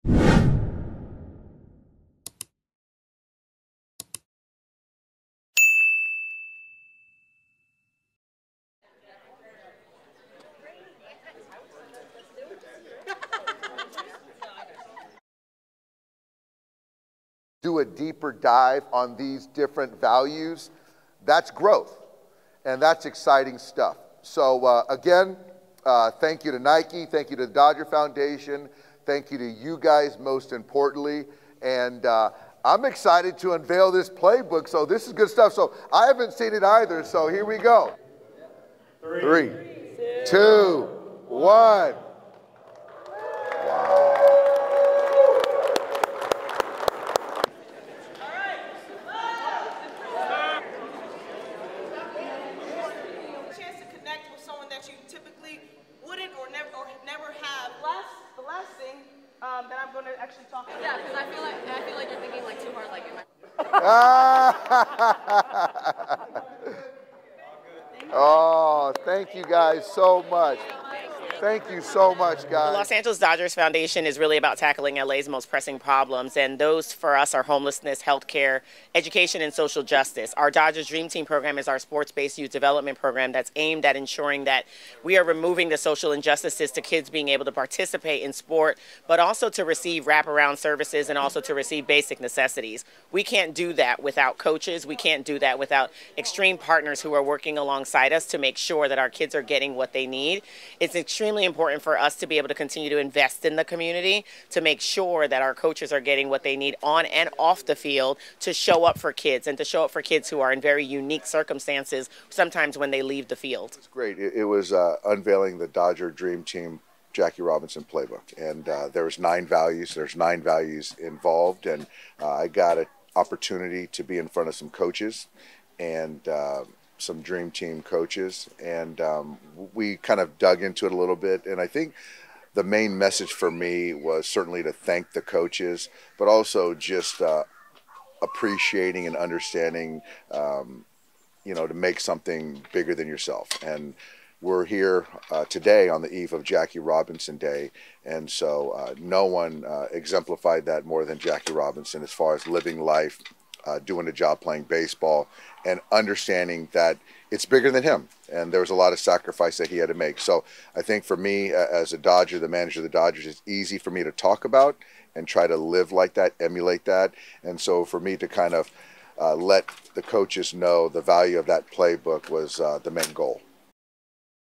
Dave Roberts, Los Angeles Dodgers Foundation unveil Jackie Robinson-inspired coaches playbook
Los Angeles Dodgers manager Dave Roberts joined the Los Angeles Dodgers Foundation for the unveiling of a coaches playbook that is inpsired by Jackie Robinson.